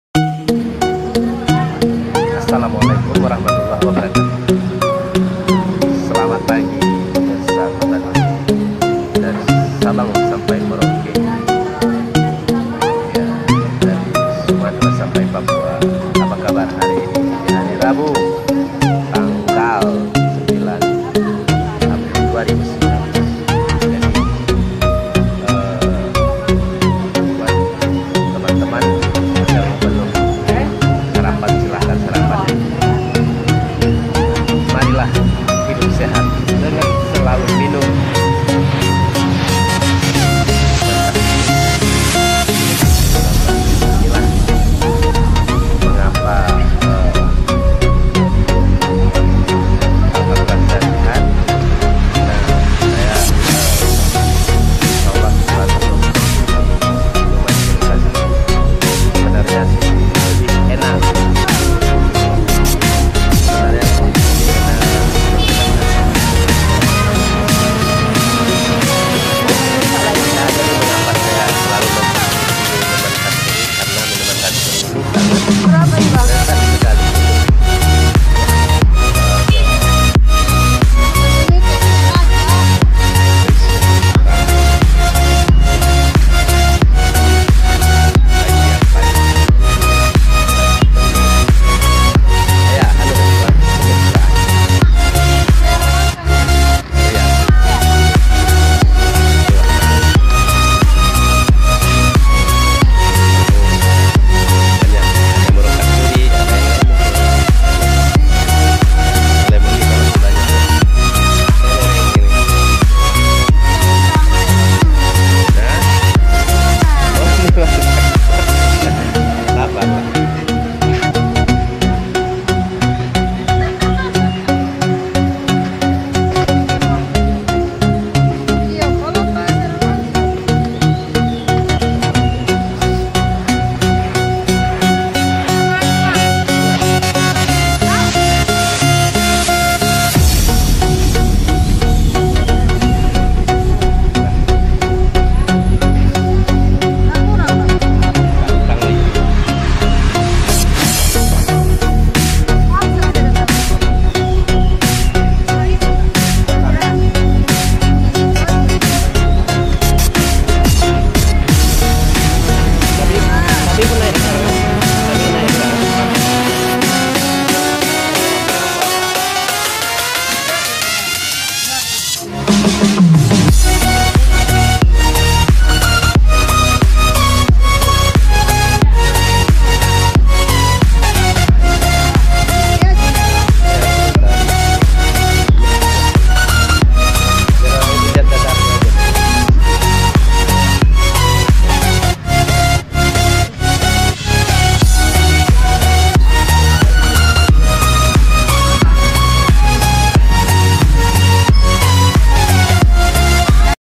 Dailyvlog bg zakaria Aceh promo minuman kesehatan es kasturi dan lemon selasa 09/04/2019
Zakaria Aceh Group merupakan distributor utama jeruk lemon dan kasturi untuk kota duri riau